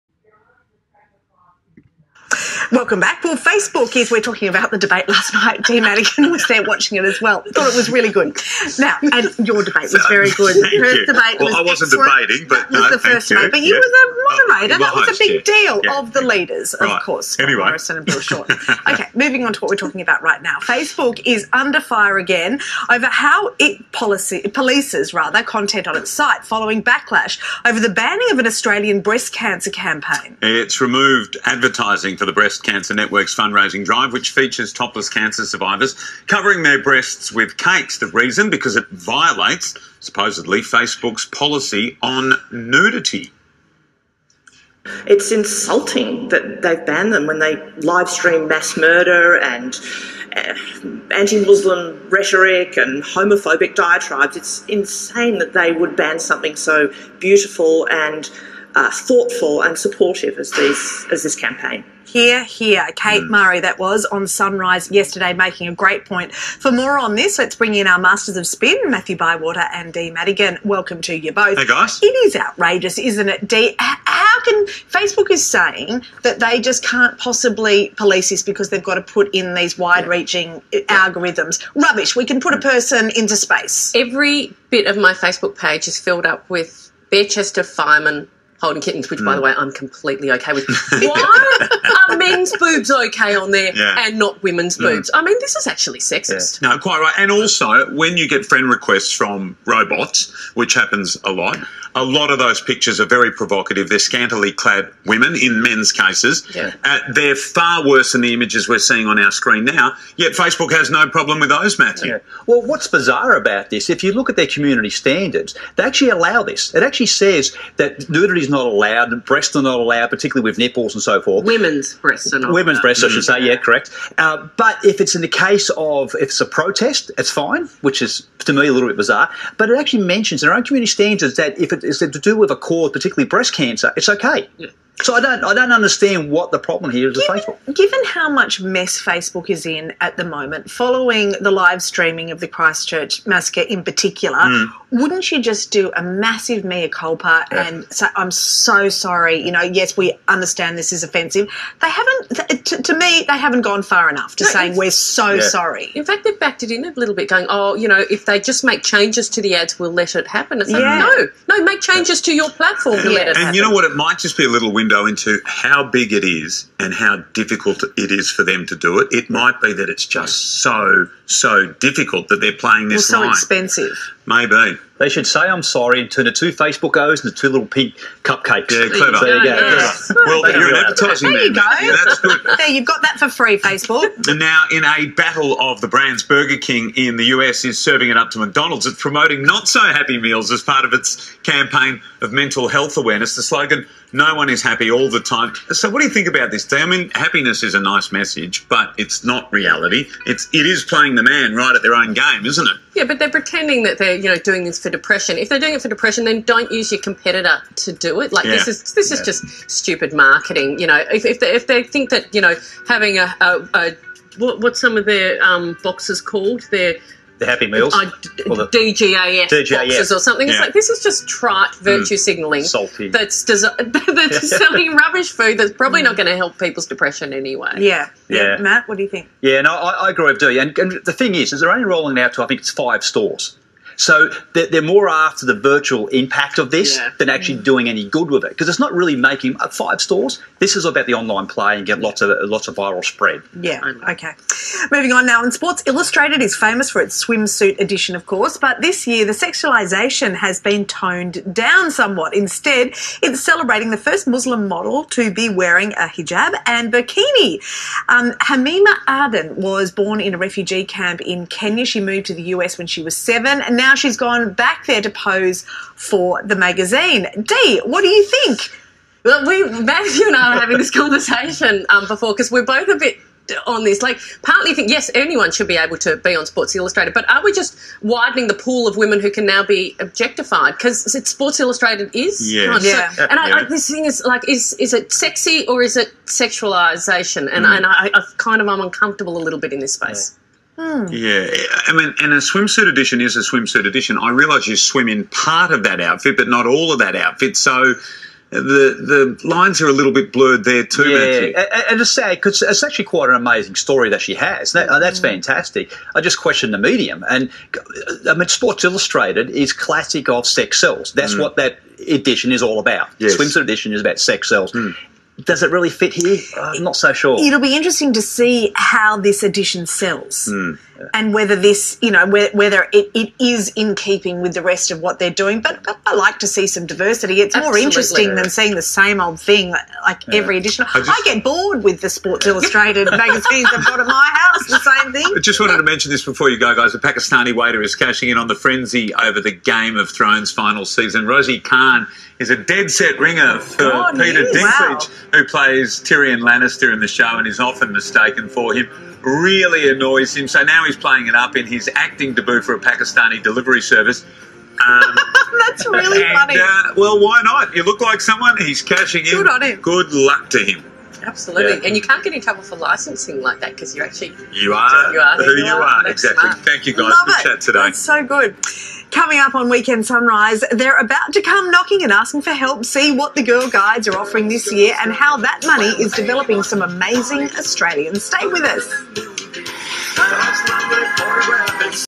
2019-5-4 Masters Of Spin
Facebook bans Breast Cancer Awareness Ad, Burger King has a sad shot at McDonalds, Sports Illustrated goes burkini